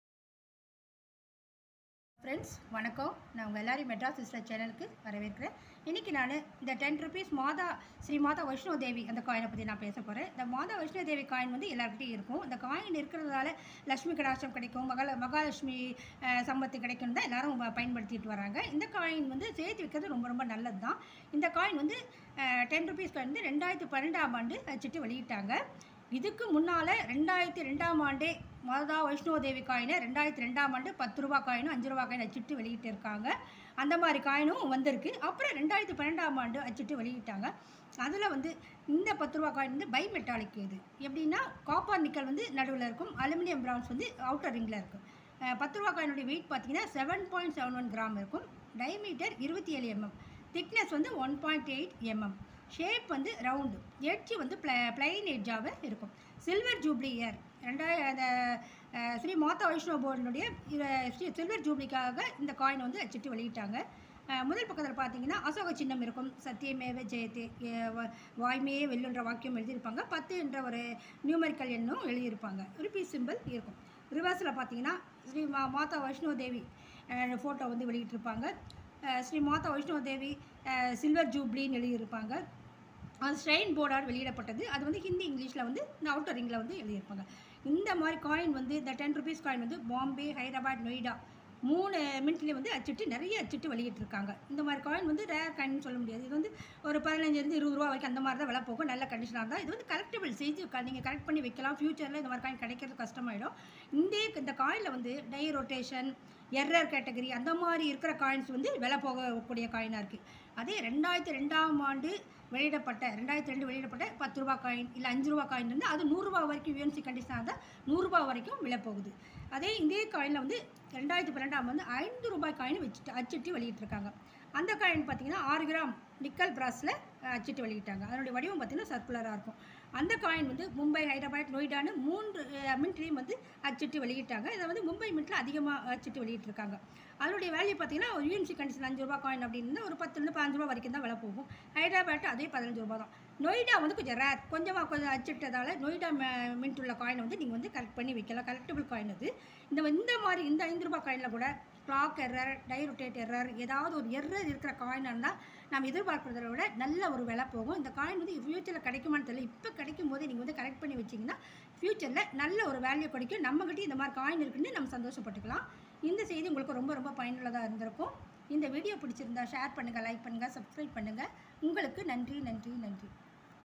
2012 ! sri vaishnavi devi coin !! ஸ்ரீ வைஷ்ணவி தேவி! 10!5! Rupee coin value !!
2012 - sri vaishnavi devi silver jublee coin.

Metal - bi - metalic copper nickal.( center - copper nickal,  outer - Aluminium bronze)

weight - 7.71 gram. dia - 27mm. Thickness - 1.8mm. shape - Round. Edge - plain.

minted - bombay,  hydreabad, noida.

value 15rs to 25rs only. more minted.

2002 minted - 5 or 10 rs coin - value 100Rs.

noida collectable. future more valuable.
any error in 5 rs coin - more value.